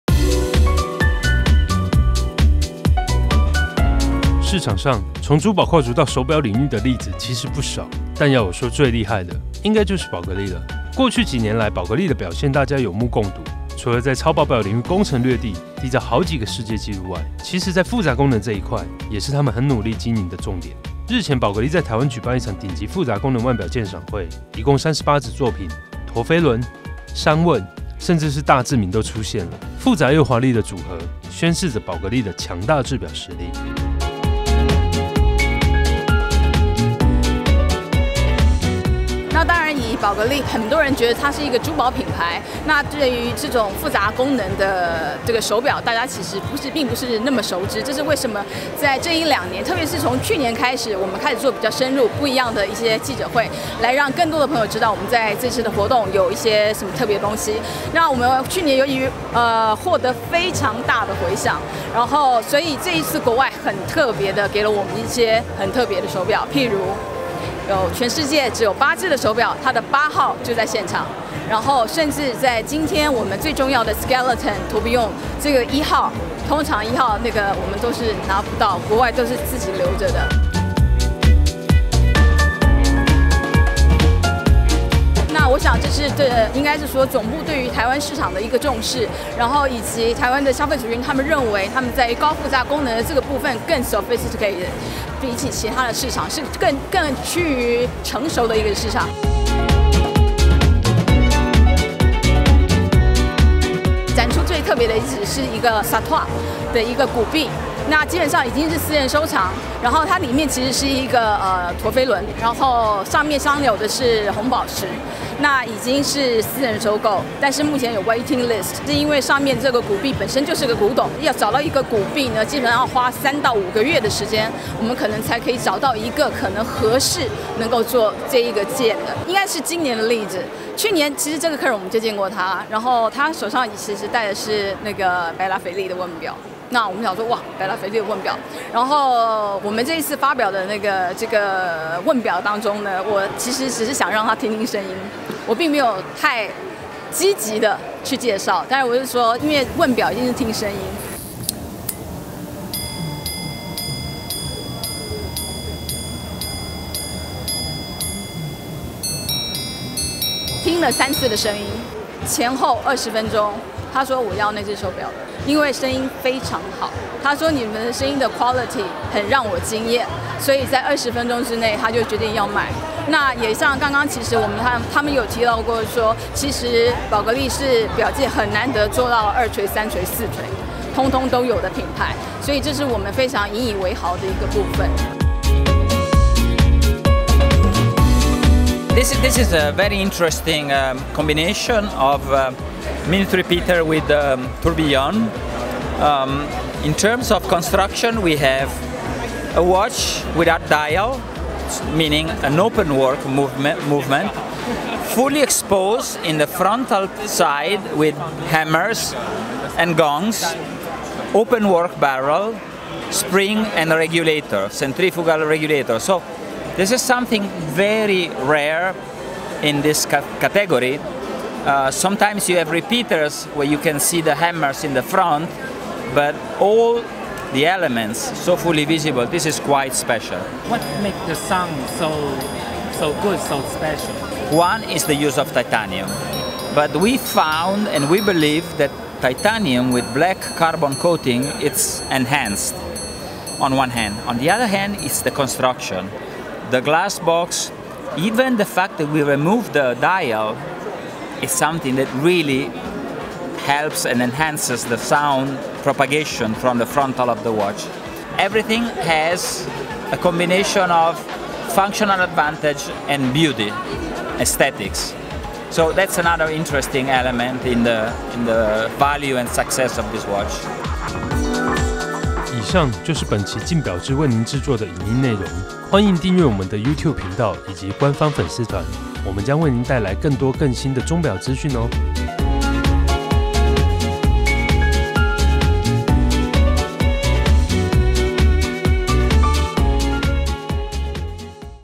華麗與複雜的巔峰組合！寶格麗2017複雜腕錶大展
近幾年寶格麗在超薄製錶的表現實在亮眼，屢屢締造世界紀錄的優異成績讓人印象深刻；另一方面，寶格麗在大複雜功能也是相當厲害，日前品牌舉辦的一場頂級複雜功能腕錶展覽中，包括陀飛輪、三問報時，甚至是最難作的大自鳴就來了數款，宣示寶格麗在複雜製錶工藝的造詣。